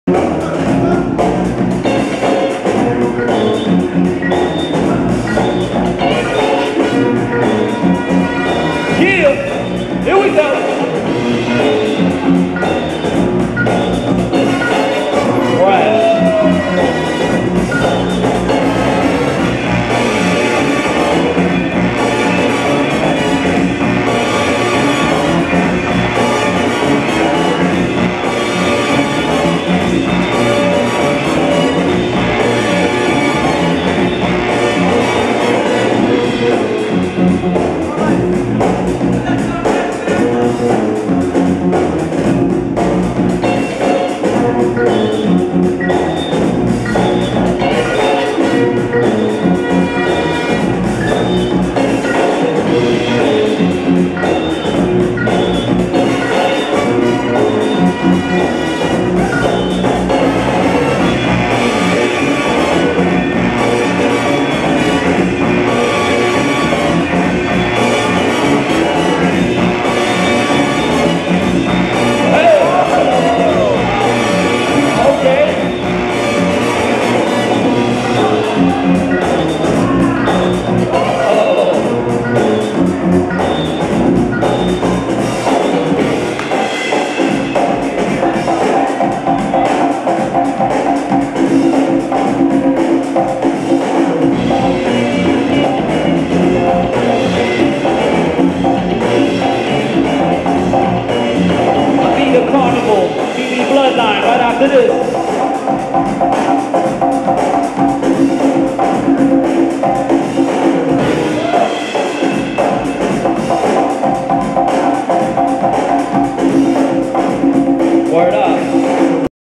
Circa 2017: (Prelims) #10 | UDEF Pro Breaking Tour
(Prelims) Circa "Pro Breaking Tour" 2017 2v2 B-Boy Battles in Las Vegas 4/1/17.